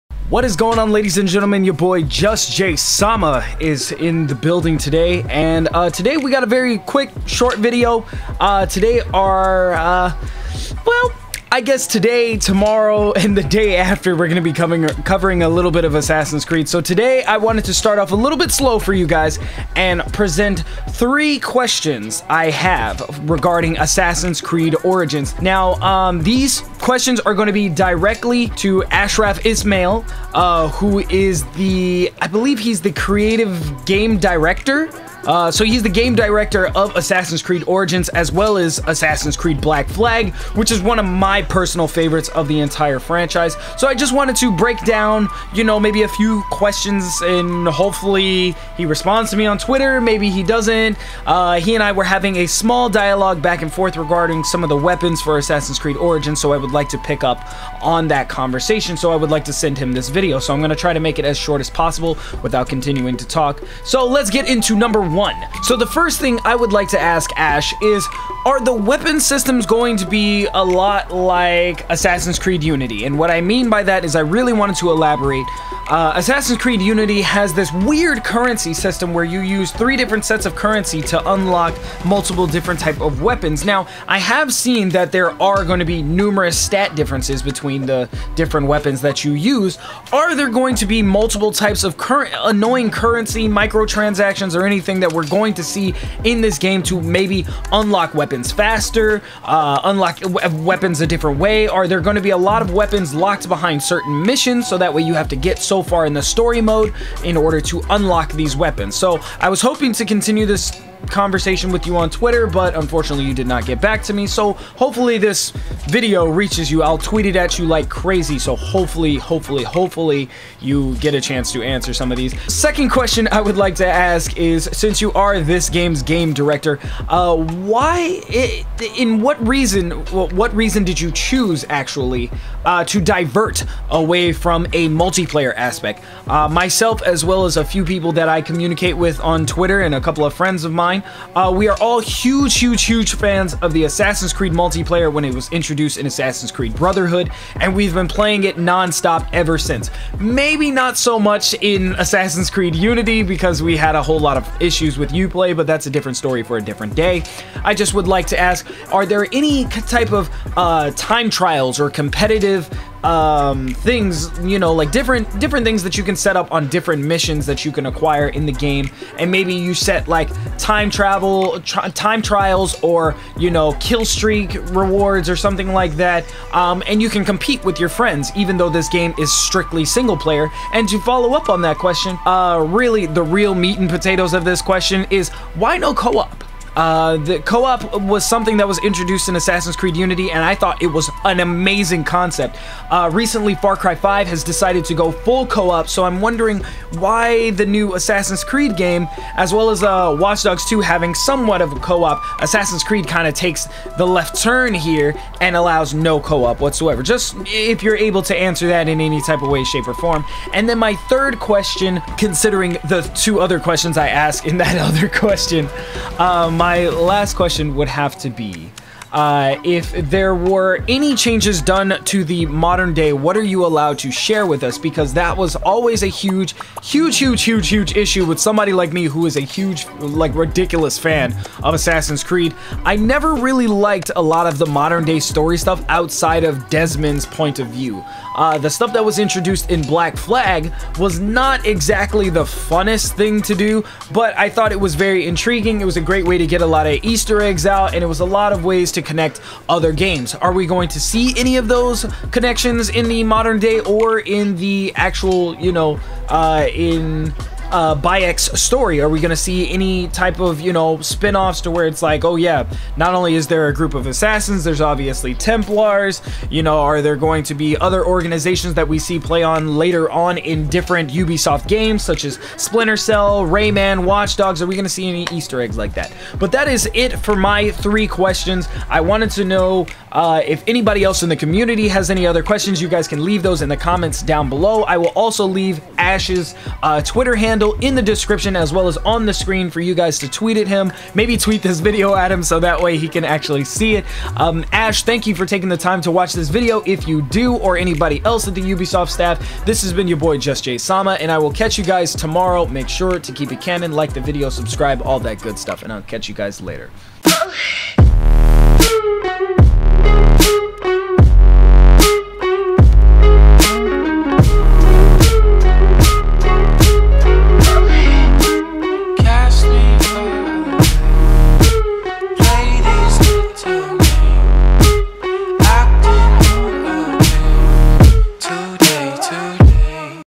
3 BURNING Questions on Assassin's Creed: Origins
I have 3 HUGE Questions regarding Assassin's Creed Origins and it I would like to direct this video to the Game Director Ashraf Ismail, who was also the Game Director on Assassin's Creed IV: Black Flag, hopefully he can get back to me about any of these questions. Assassin's Creed Origins is looking to be one of the best in the franchise so far.
If you would like to Tweet this to Ash here is his Twitter:

►Hit me up on Instagram or Twitter